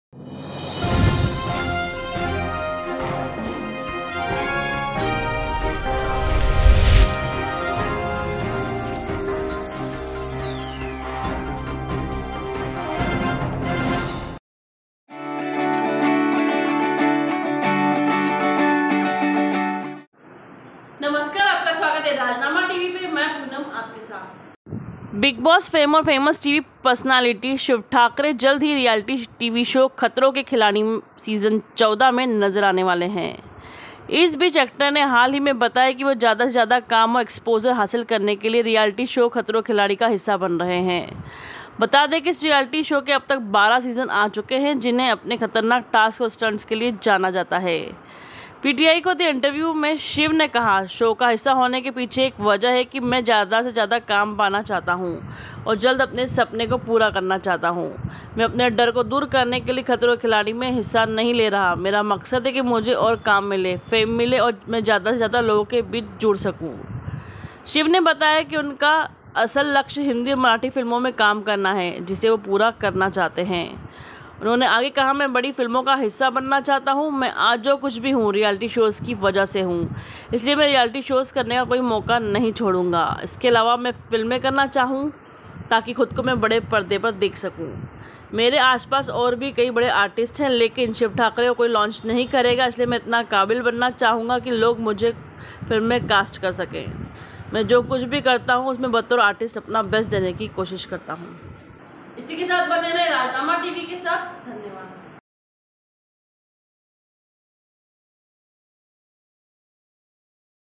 anchorpoonam शिव ठाकरे ने बताई 'खतरों के खिलाड़ी' करने की वजह
About this video:शिव ठाकरे ने बताई 'खतरों के खिलाड़ी' करने की वजह